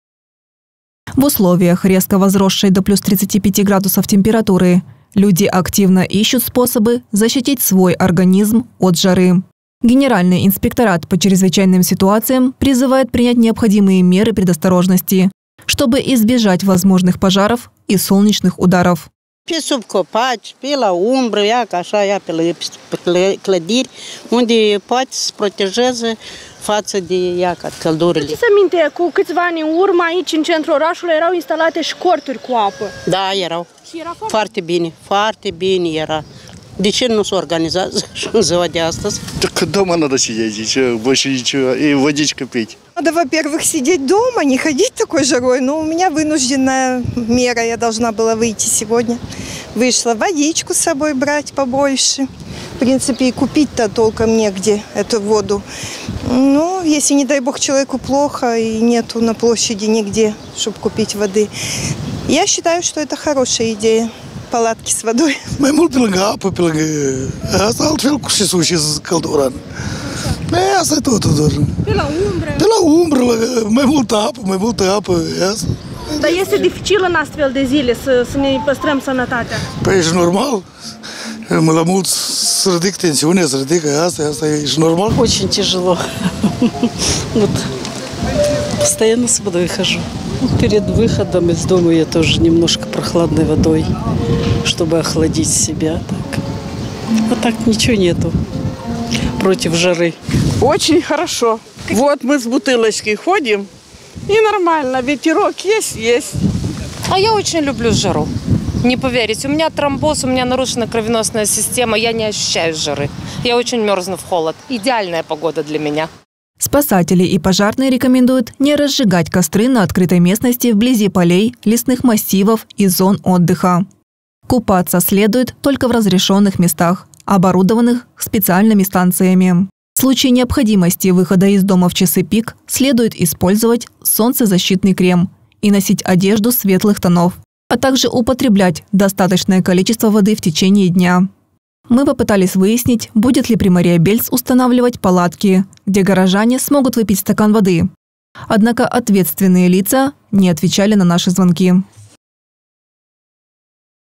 ИЮЛЬ НАЧАЛСЯ С РЕКОРДНЫХ ТЕМПЕРАТУР
В условиях резко возросшей до +35°C температуры, люди активно ищут способы защиты организма от жары. Генеральный инспекторат по чрезвычайным ситуациям призывает принять необходимые меры предосторожности, чтобы избежать возможных пожаров и солнечных ударов.